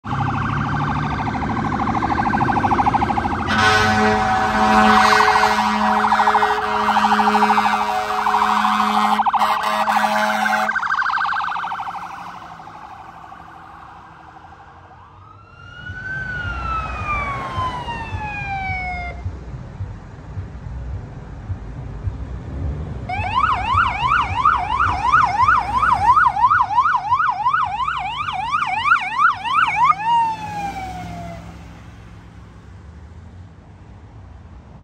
Middletown EMS Ambulance 31-4-56 & First Responder 31-4-55 Responding 10/20/22
Location: Middletown NJ
Call Type:  Medical
Units In Video: Ambulance 31-4-56 & First Responder 31-4-55
Date Filmed: 10/20/22
Additional Info: N/A